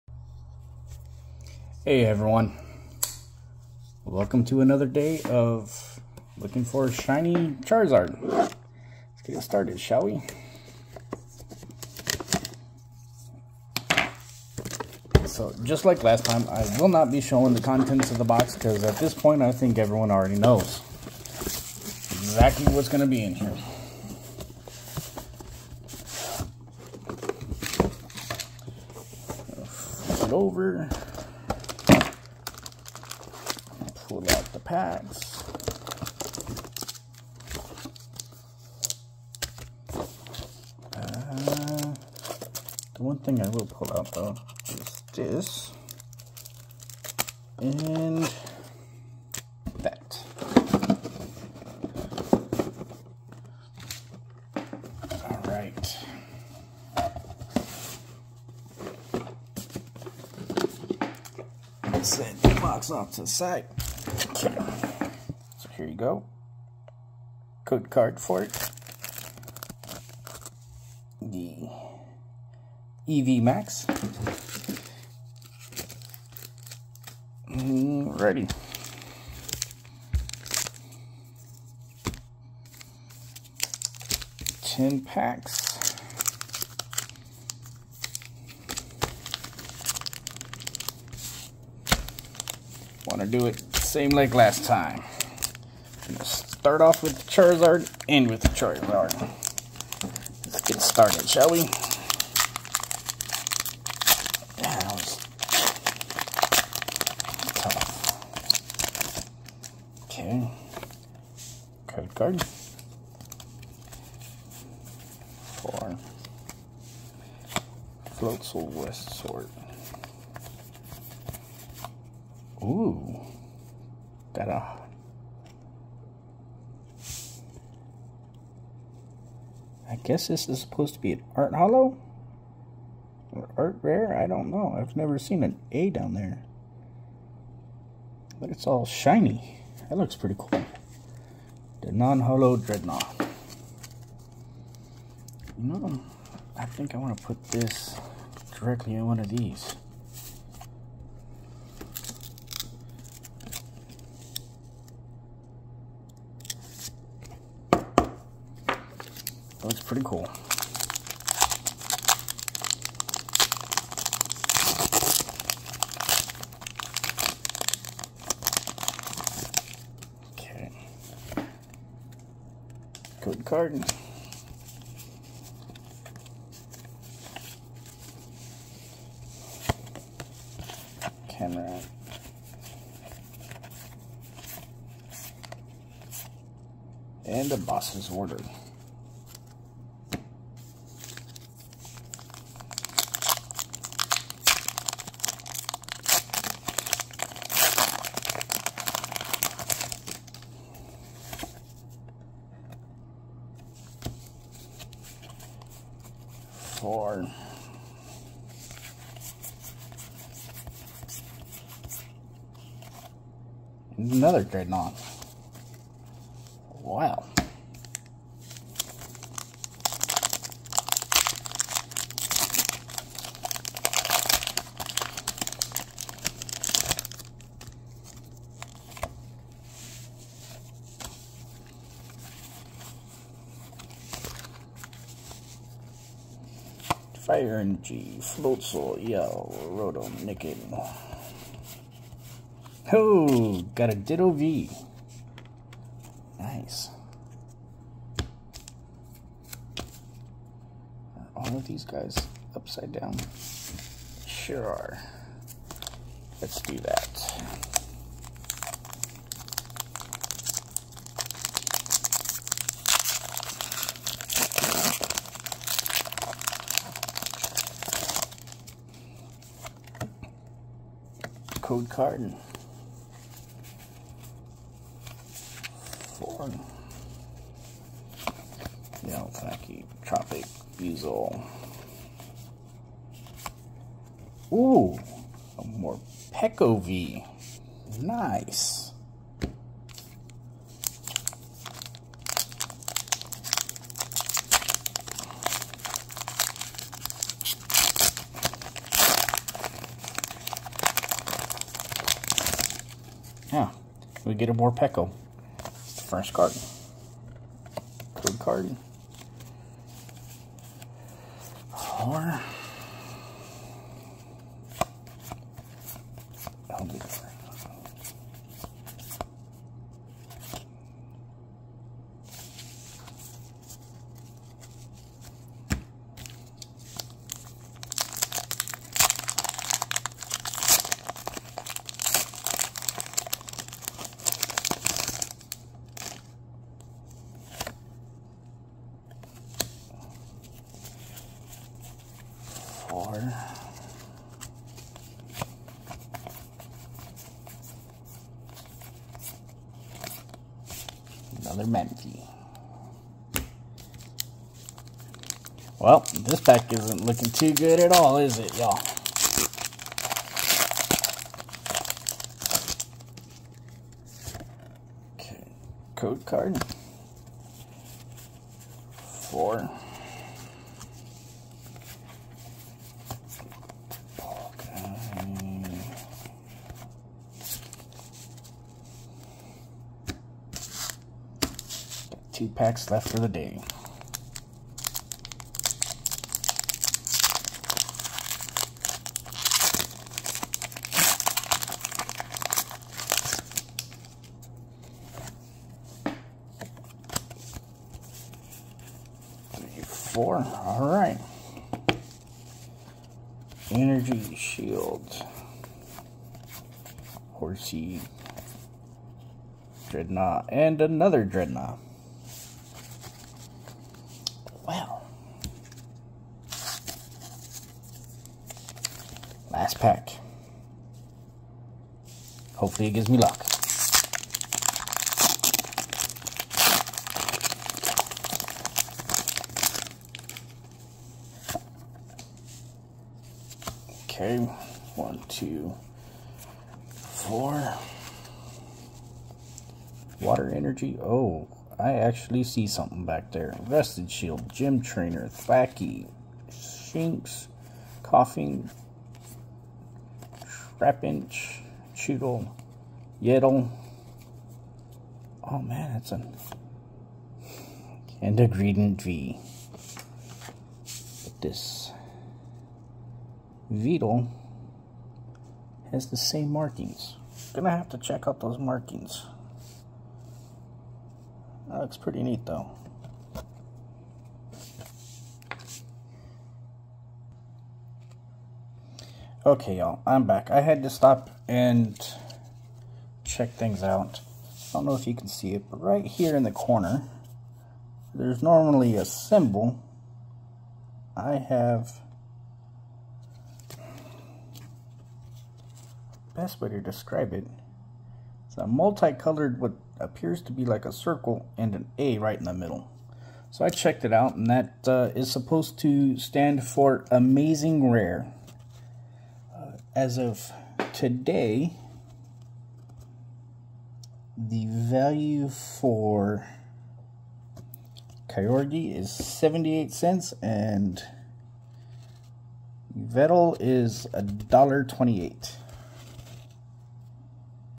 Charizard Hunt #4
Our fourth video trying to capture that Charizard. Will he still escape our grasp?